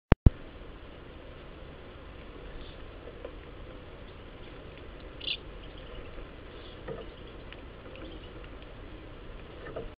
Voederhuisje_met_camera.mp4
Vogel voederhuisje met camera in actie. Verkrijgbaar op